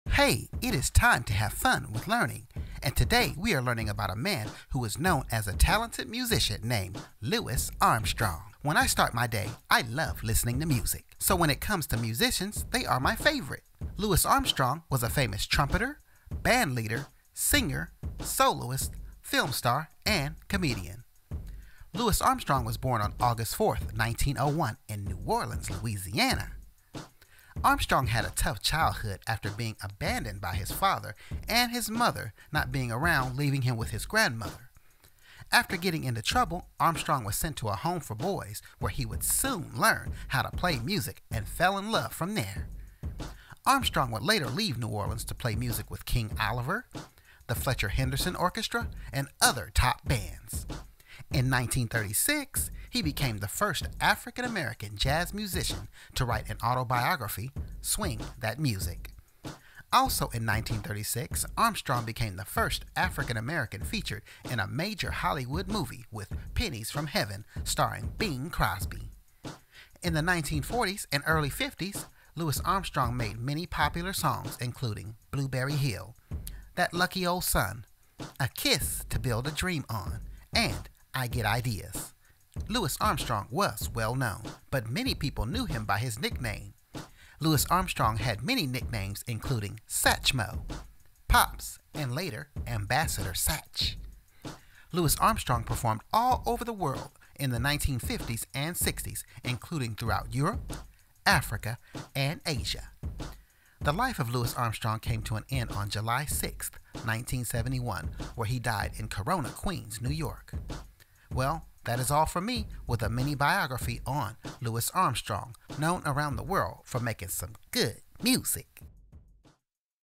Celebrate Black History Month: Louis Armstrong Biography (Black History Educational Videos)
Celebrating Black History Month Video watch this Louis Armstrong Biography. Come and learn about a famous trumpeter, singer, and actor. In the world of jazz, Louis Armstrong is legendary and made his mark on the world of music and film.